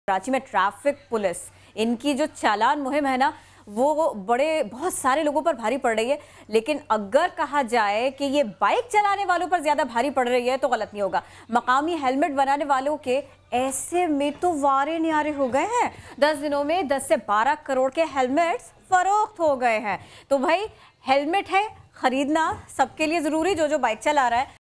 10 din mein 10 crore ke helmet farokht | SAMAA TV | 08 July 2019
ٹریفک پولیس کے چالان، دس دن میں دس کروڑکے ہیلمٹ فروخت